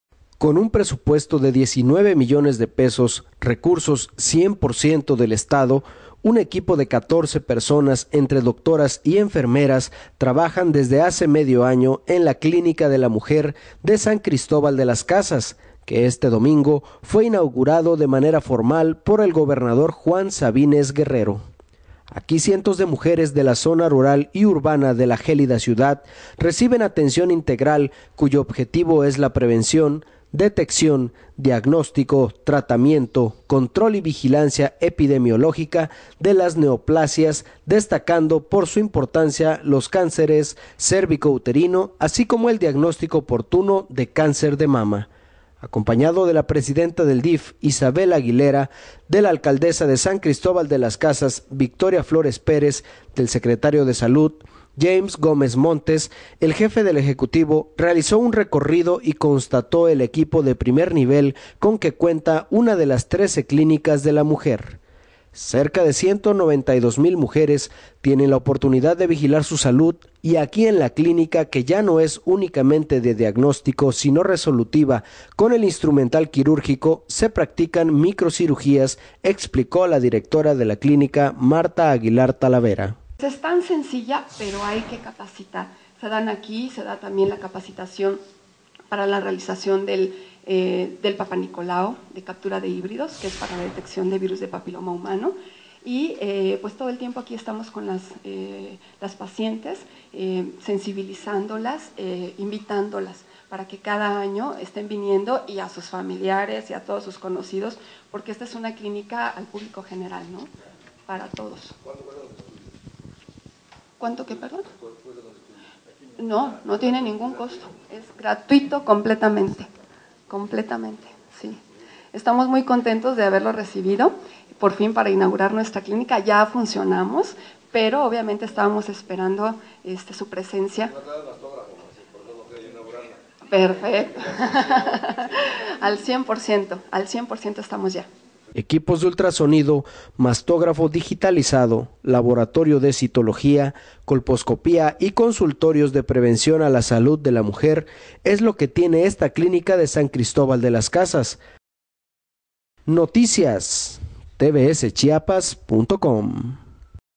TVS Chiapas.- Gobernador pone en marcha Clinica de la Mujer No. 13, San Cristobal de las Casas
- Gobernador Juan Sabines pone en marcha Clinica de la Mujer No. 13, San Cristobal de las Casas